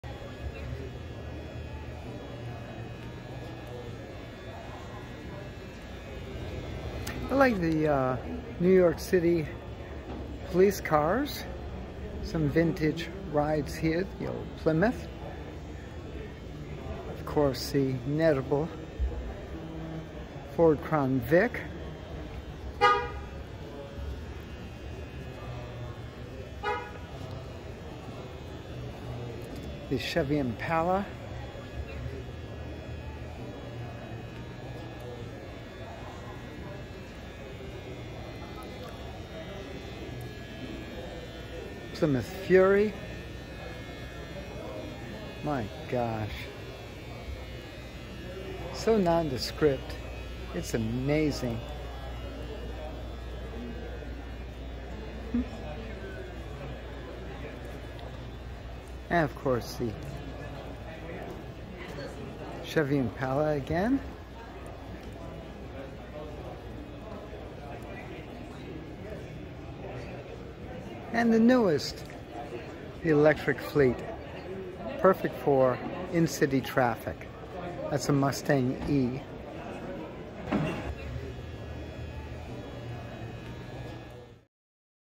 Vintage New York City Police Cars 2024 New York International Auto Show
Walk along the progession from the 60's through today's New York City police cars at the 2024 New York International Auto Show.  For more photos, videos and your regional calendar of events,